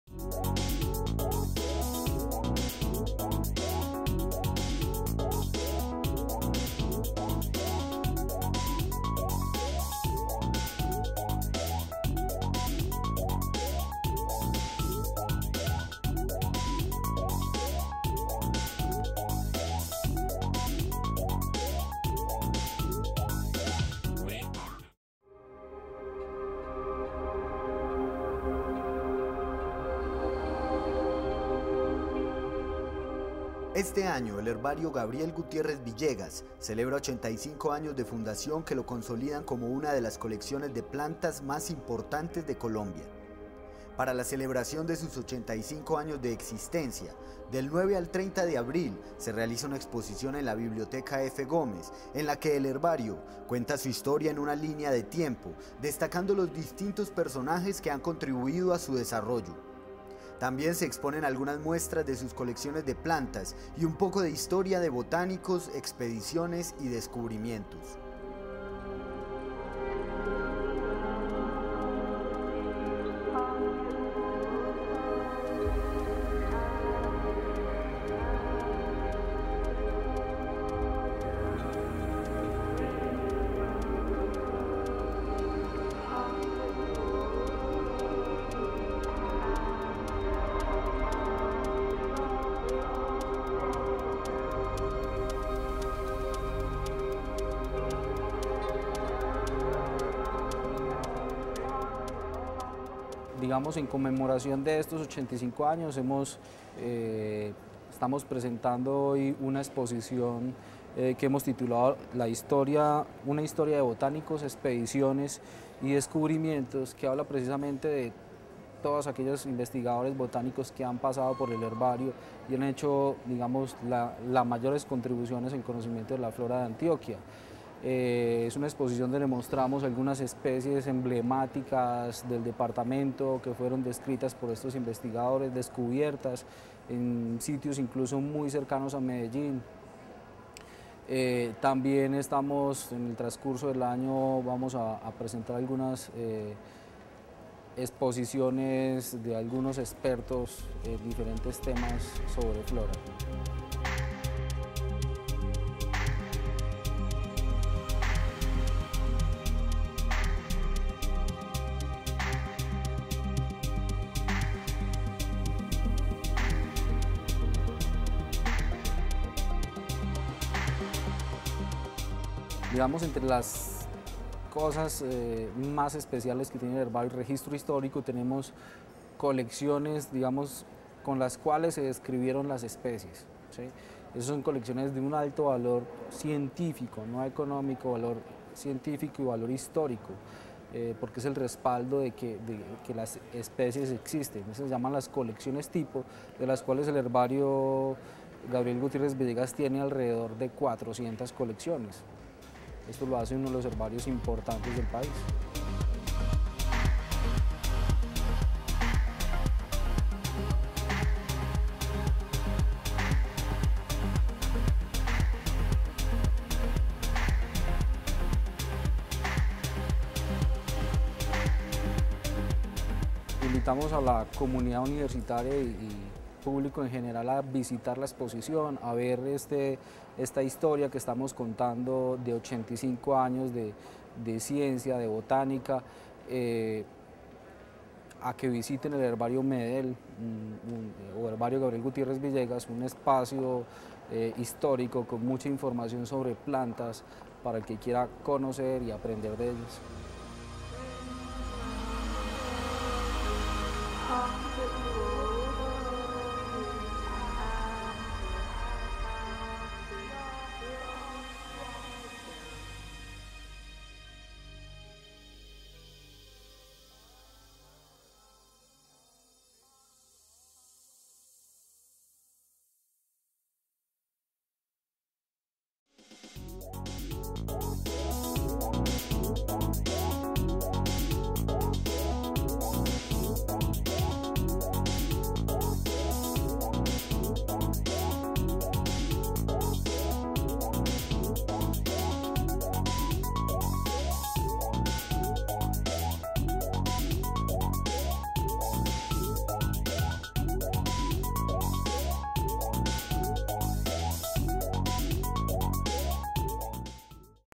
85 años Herbario MEDEL
Este año el Herbario Gabriel Gutiérrez Villegas celebró 85 años de fundación que lo consolidan como una de las colecciones de plantas más importante de Colombia.  Para la celebración de sus 85 años de existencia,  del 9 al 30 de abril se realizó una exposición en la biblioteca Efe Gómez, en la que cuenta su historia en una línea de tiempo, destacando los distintos personajes que han contribuido a su desarrollo. También se expusieron algunas muestras de sus colecciones de plantas, y un poco de historia de botánicos, expediciones y descubrimientos.